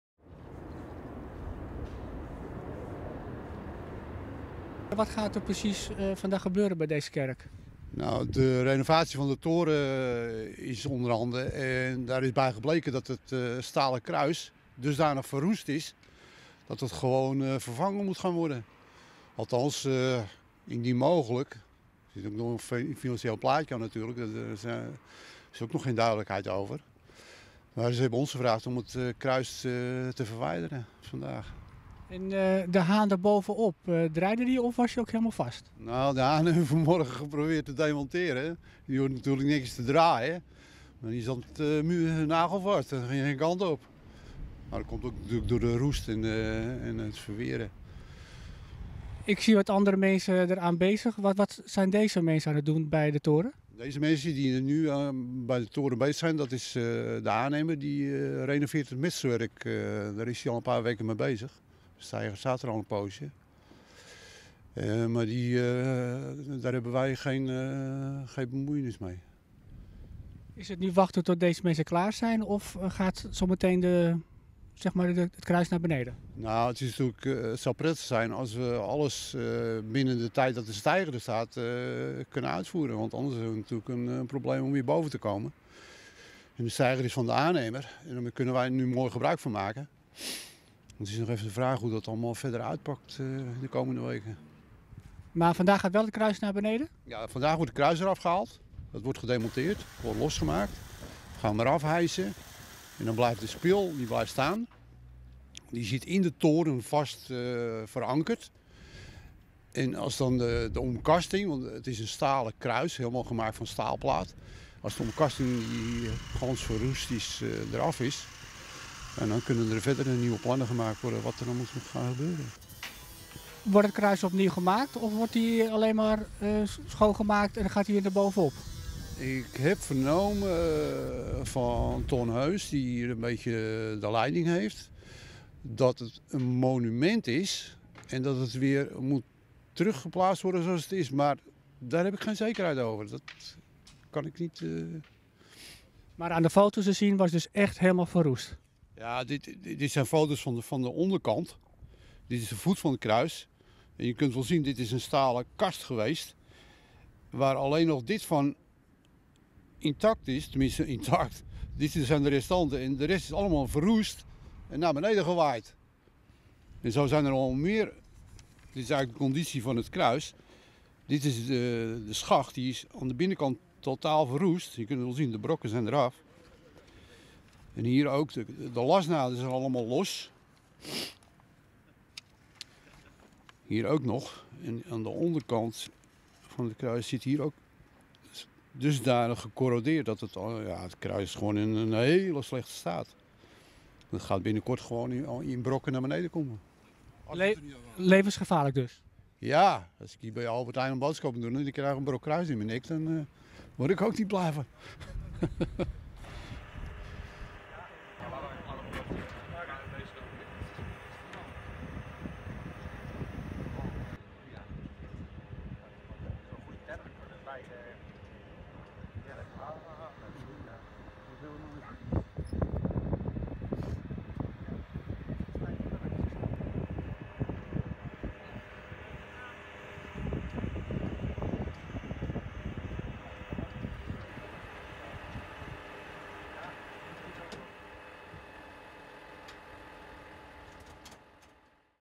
Kruis van Mariakerk wordt gerenoveerd
De kerktoren van de Mariakerk aan de Spoellaan in Heemskerk staat in de steigers voor renovatie. Daar hoort ook het stalen kruis bij. Het kruis is verroest en dient vervangen te worden, aangezien het anders tot gevaarlijke situaties kan leiden.